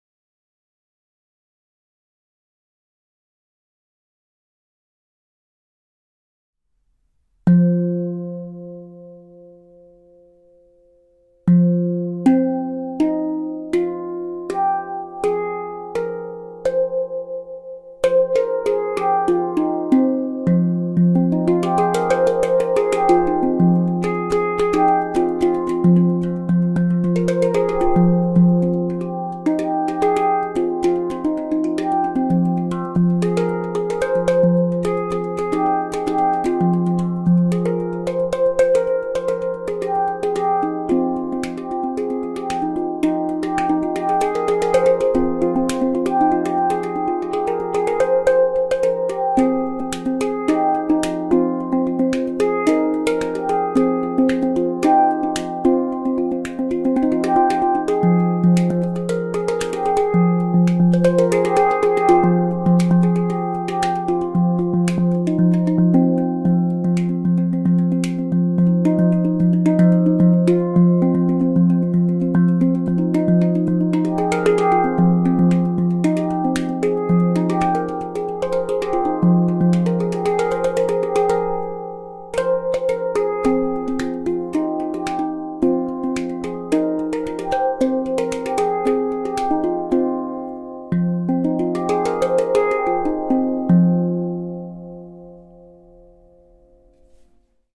Aura "F Celtic Minor 7" - Resonance Series
Scale: (F), C, Eb, F, G, Ab, Bb, C
# 511
Order this scale here:

The Resonance Series Auras are crafted out of stainless steel, which, after several rounds of heating, becomes a distinctive earthy orange-brown color. Stainless steel provides the Resonance Series with increased sustain and warm timbre, while also lessening the impact sound when striking notes. The increased sustain makes the Resonance Series instruments perfect for more meditative play, where letting notes ring out is ideal. Stainless steel provides inherent corrosion resistance.